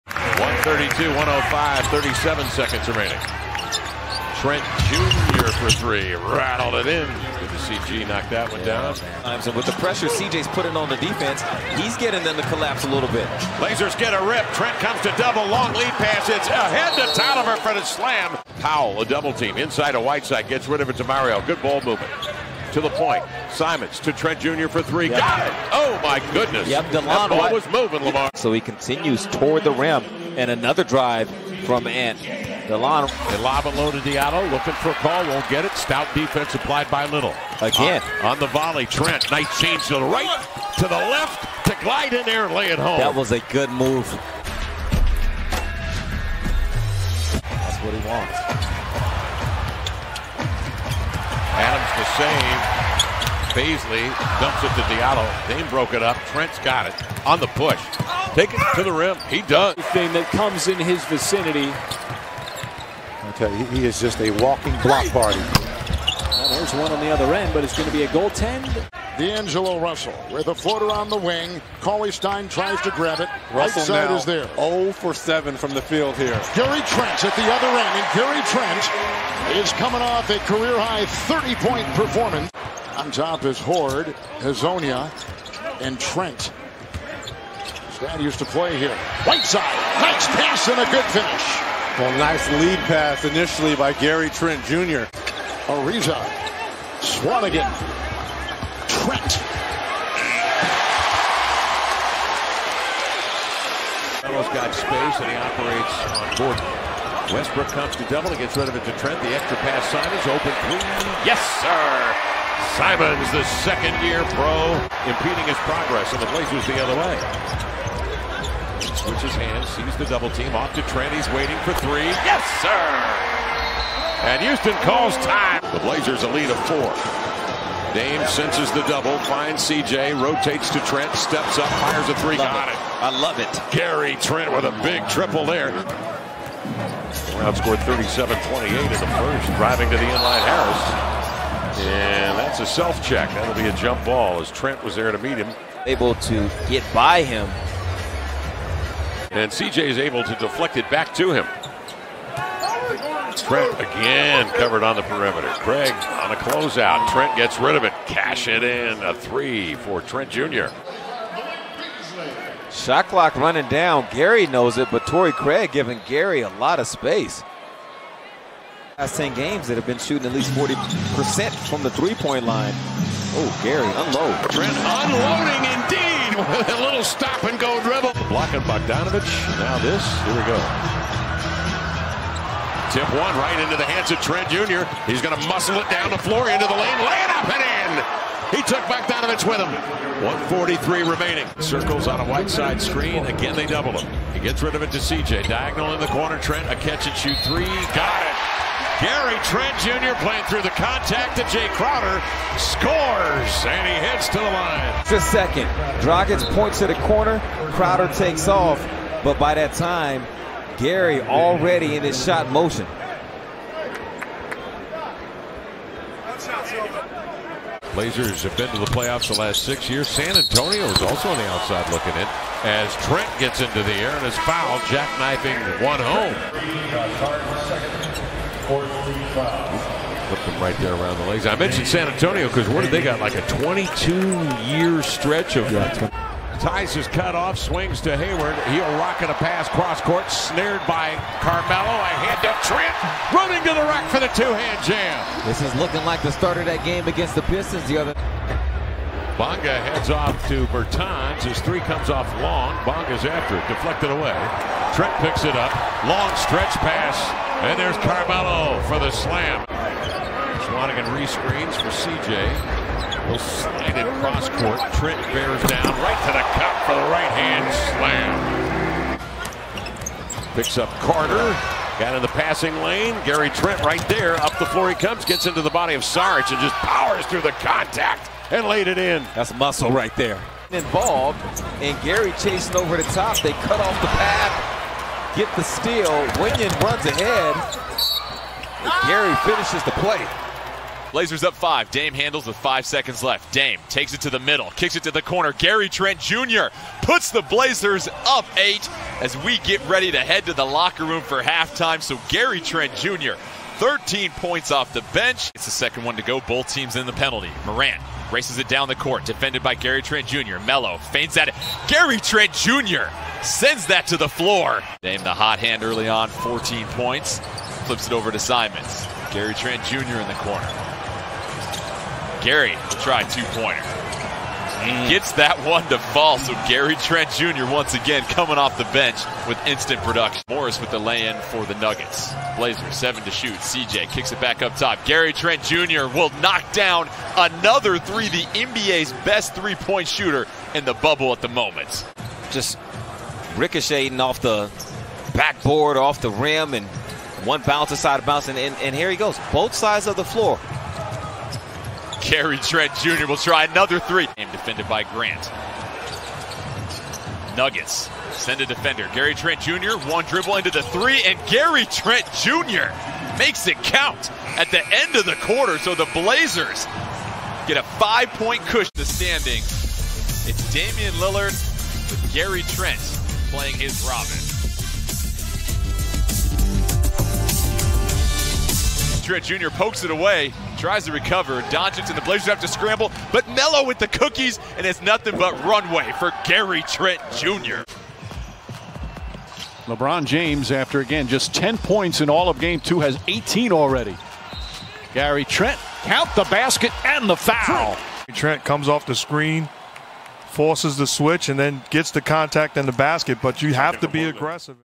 Gary Trent Jr.'s Top Plays from the 2019-20 Season | Trail Blazers Highlights
Maybe THE breakout player of the bubble, GTJ blossomed this season into a sharpshooter and tenacious defender.